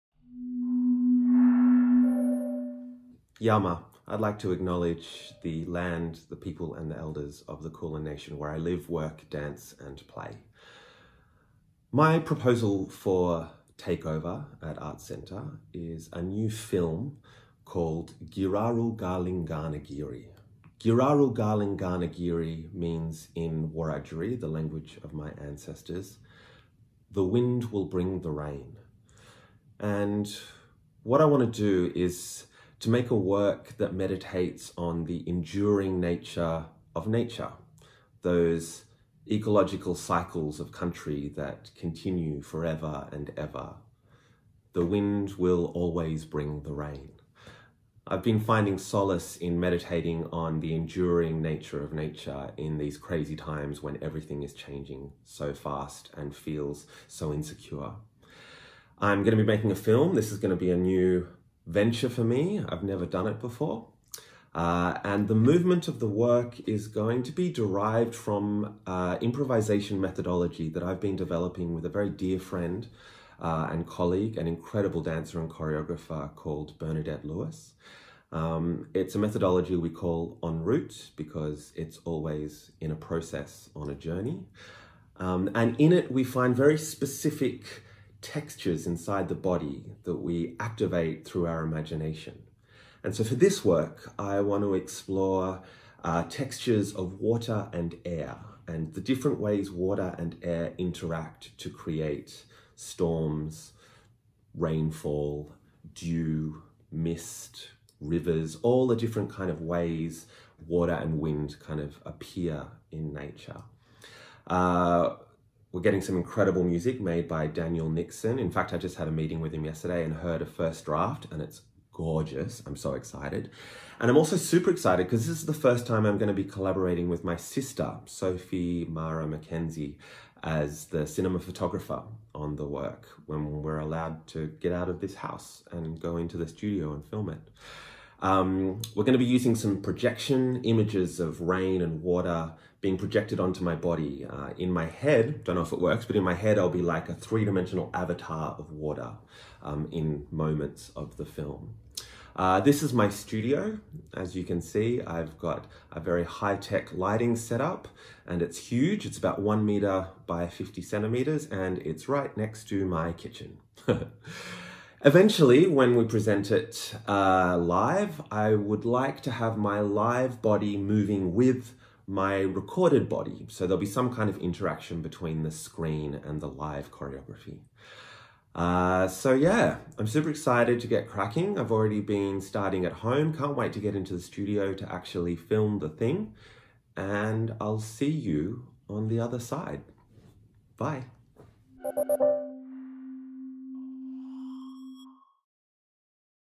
Take Over! | Introducing Joel Bray
Giraru Galing Ganhagirri
An installation weaving the dancing body with the digital image, reflecting on the endurance of Country. Speaking to the assurance that, in nature, one thing follows another. It always has and always will.

Giraru Galing Ganhagirri translates to “the wind will bring the rain” in the Wiradjuri language. It also speaks of the bringing together of the elements of air and water.

Take Over! is a unique partnership between Arts Centre Melbourne and Melbourne Fringe, this year’s program features 10 fearless and unique artistic explorations that will take shape in artists’ homes across Victoria in the lead up to their presentation at Melbourne Fringe in November 2020.